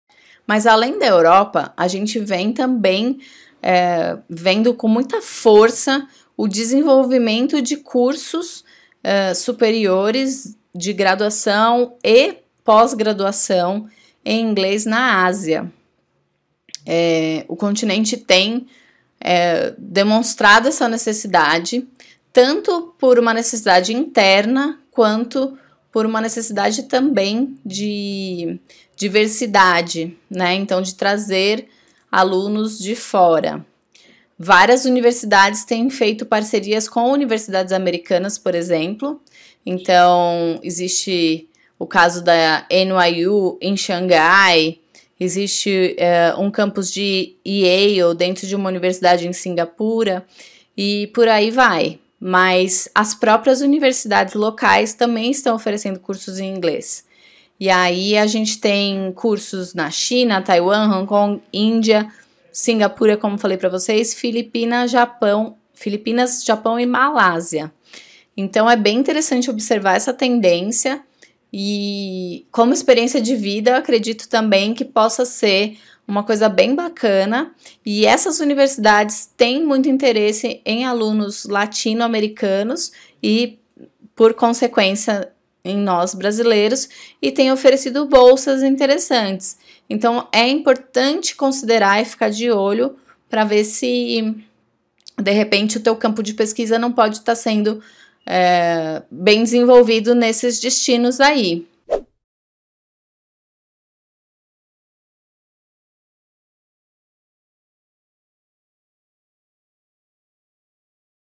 Pós-graduação em países que você deveria considerar | VIAJAR PRA ESTUDAR
Quando decidimos estudar no exterior é quase automático pensarmos em destinos mais óbvios, ou "famosos": Estados Unidos, Canadá, Reino Unido e vários outros países da Europa. Mas você já considerou algum país da Ásia? Assista ao vídeo e veja porque é bom você considerar.

Este vídeo é um trecho do vídeo "TUDO QUE VOCÊ PRECISA SABER PARA ESCOLHER SUA PÓS-GRADUAÇÃO NO EXTERIOR" e que pode ser visto na íntegra no

Para obter informações sobre minha assessoria para estudar no exterior mande um direct para o Instagram: